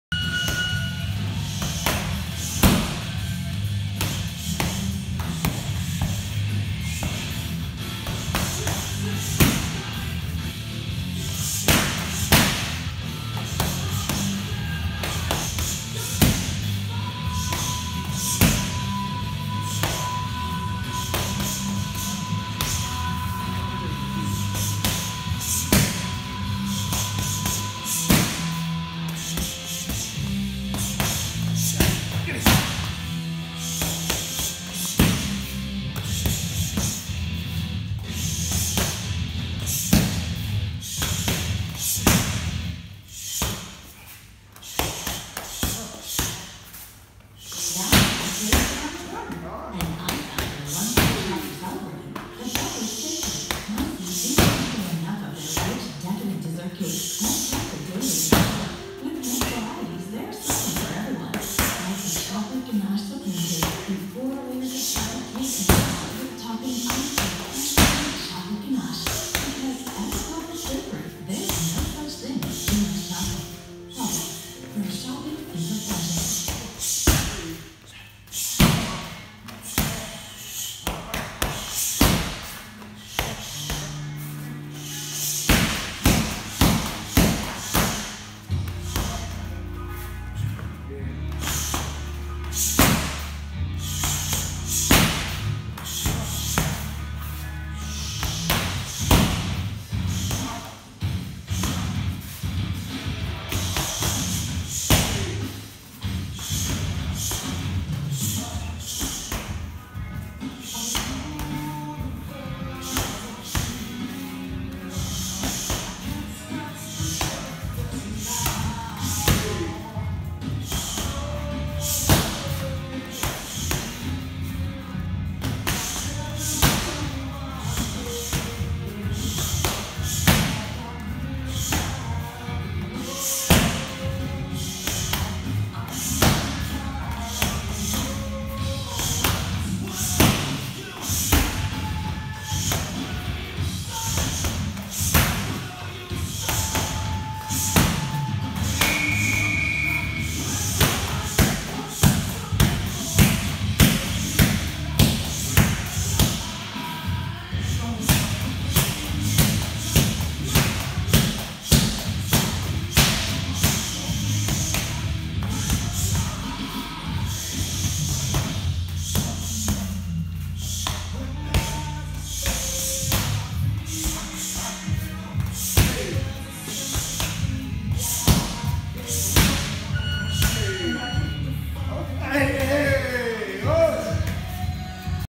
Pad Work
Getting in some pad work with Jaral Bowman